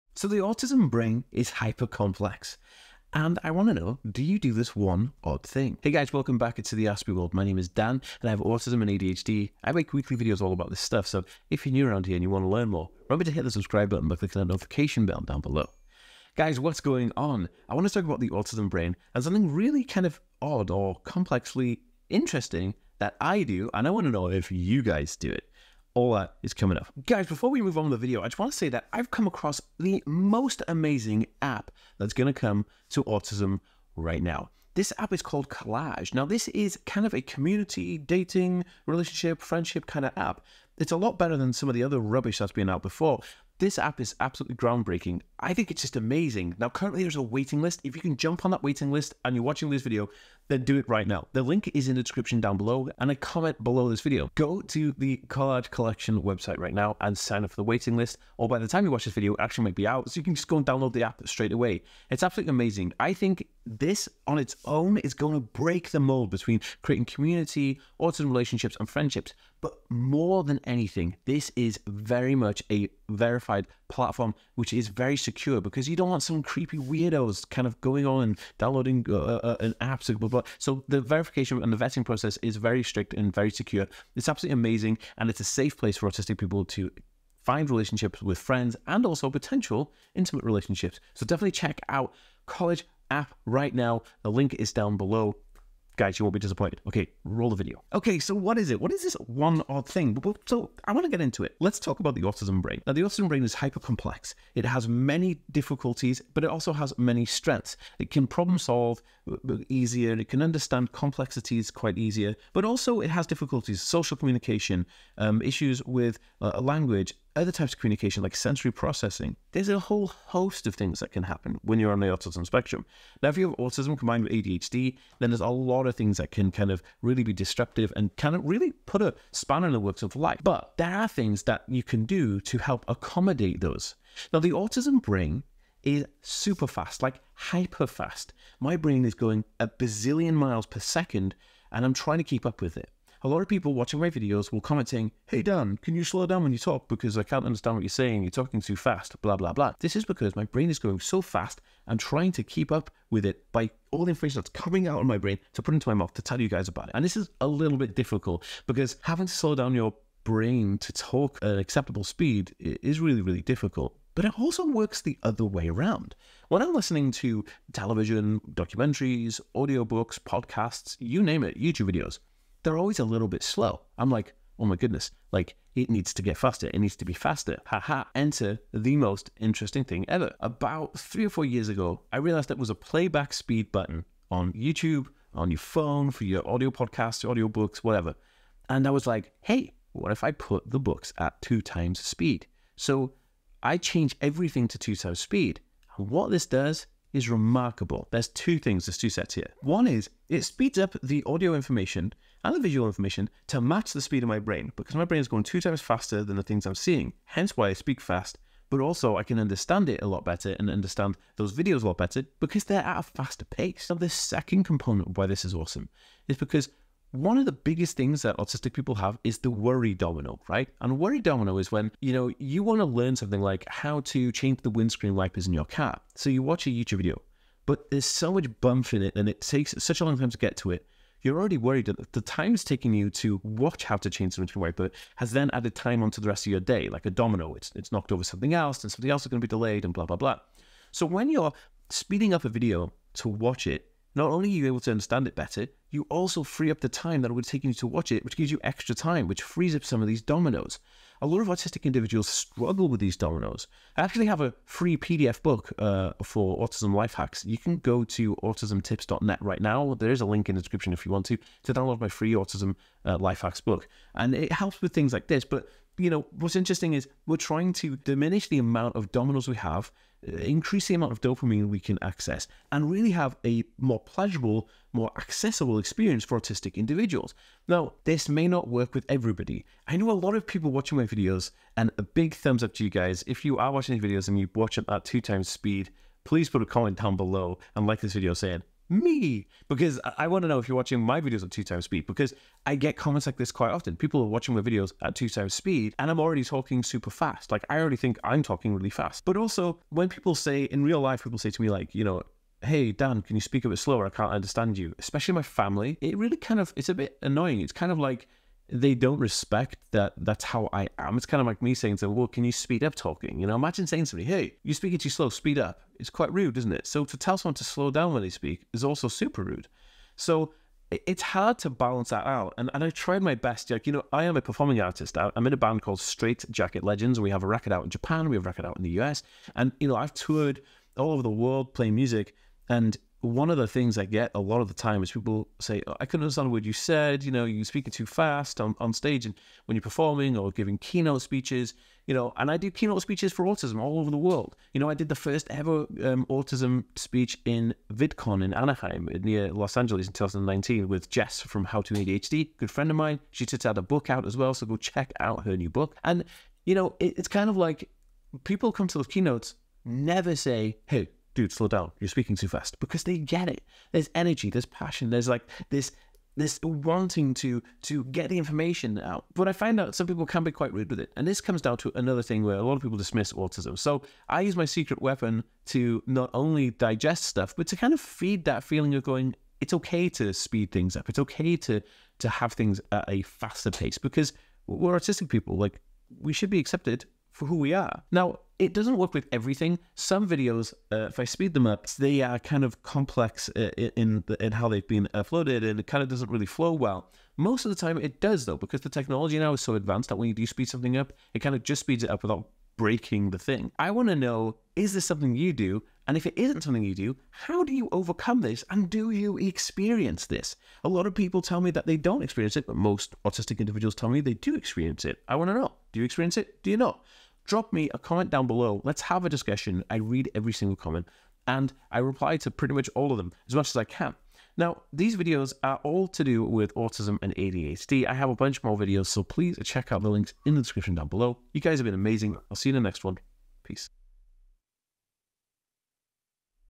Autism Brain: Do YOU Do This Odd Autistic Behaviour?
Autism Brain is complex but do you do this one interesting thing!?

Having an autism brain is a blessing sometimes and an issue other times. I love my autism brain and I don’t think I could live without my autism brain.

That said, while I was listening to a podcast in my car the other day my partner came into the car and said how on earth are you listening to that?

She was referring to the fact I had my podcast on x2 speed to listen to

People with adhd and autism have a super fast autism brain type and the faster the brain the quicker data needs to be digested.

As an autistic person I find myself bored of normal speed conversations, so I’m I’m watching a video or listening to a book or podcast I have to x2 the speed to digest the data at my level.

Do you do this? Let me know in a comment.

📌 MY BOOK Autism For Adults

If you’re reading this you’re a hero!
Comment “I am autistic!” so I can notice you!